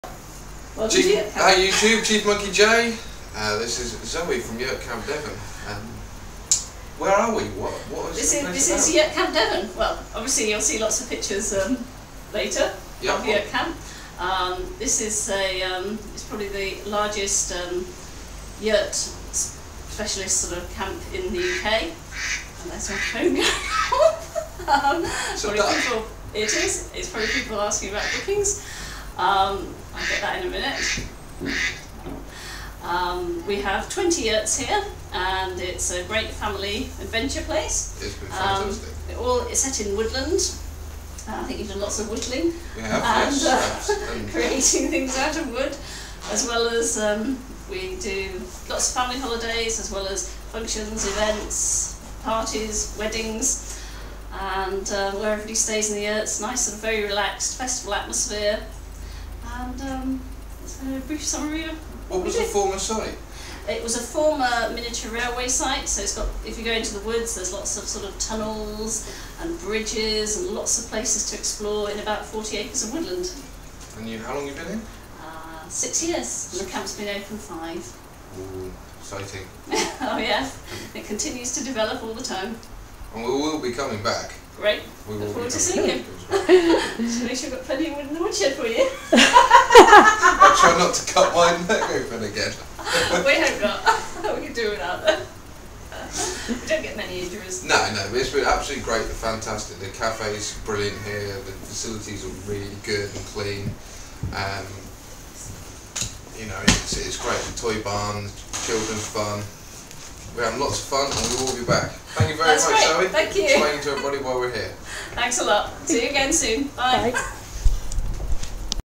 Zoe the Boss of Yurt Camp devon
Me asking Zoe the Boss of Yurt Camp Devon about the site